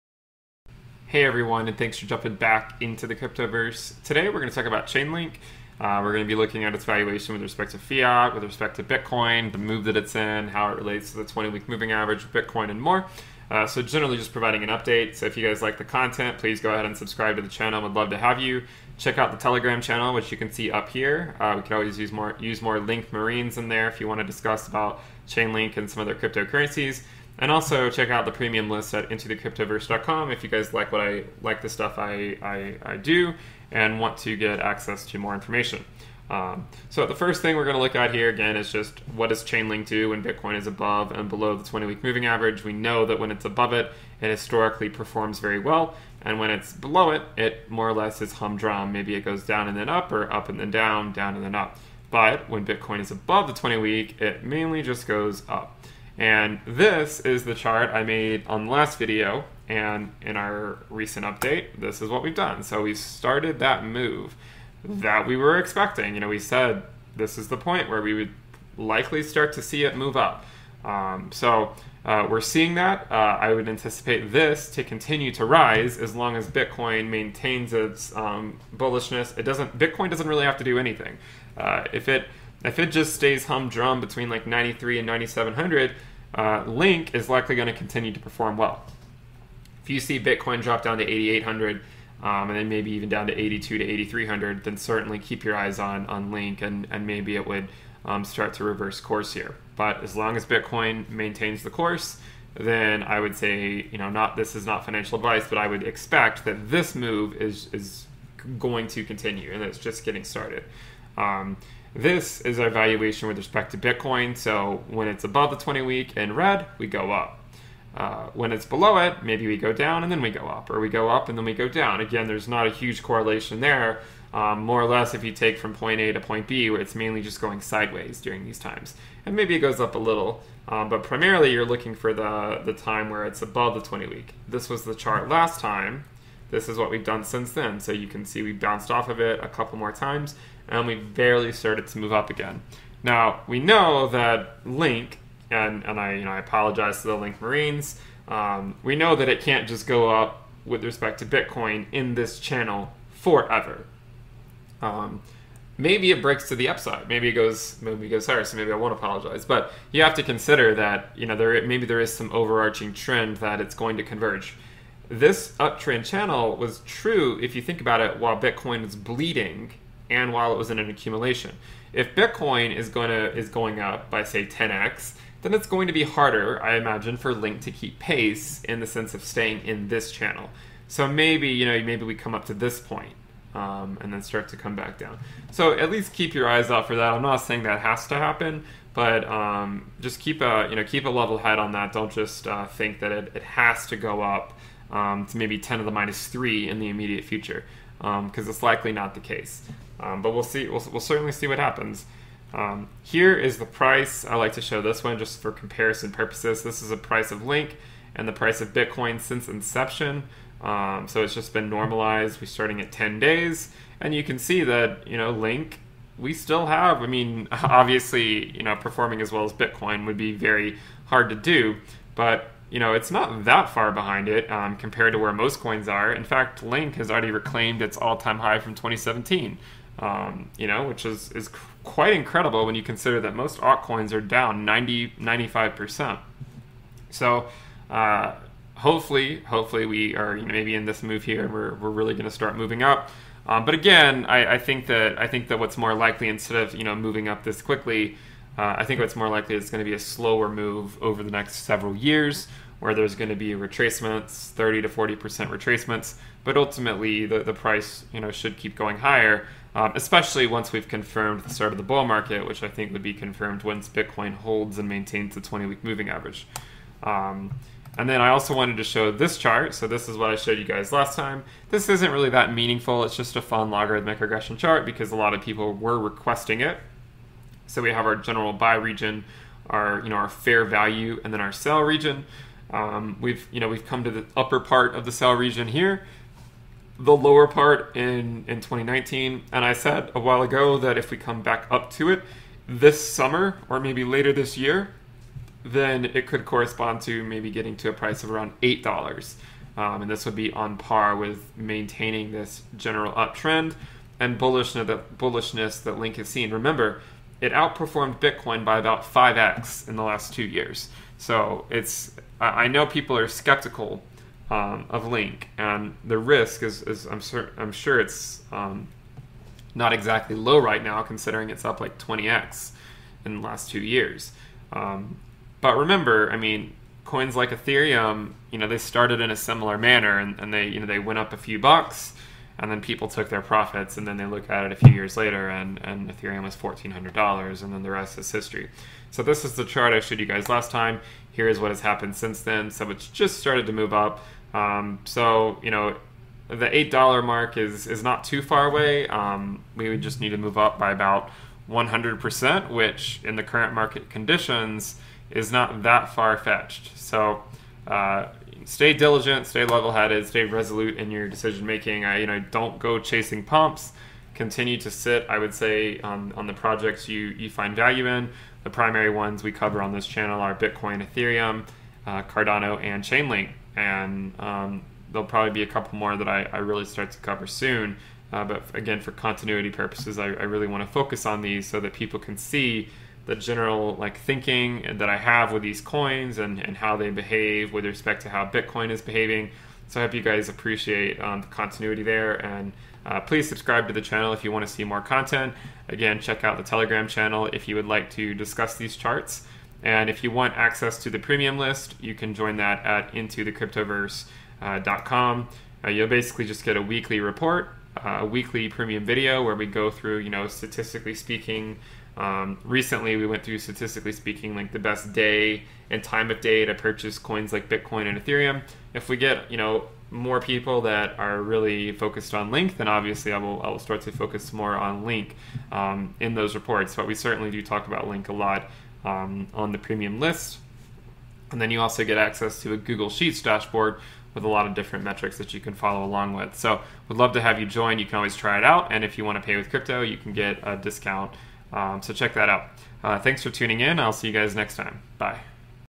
Chainlink price begins its typical breakout
Chainlink price tends to breakout when Bitcoin gets and stays above the 20 week moving average. The movement during this iteration seems to have started.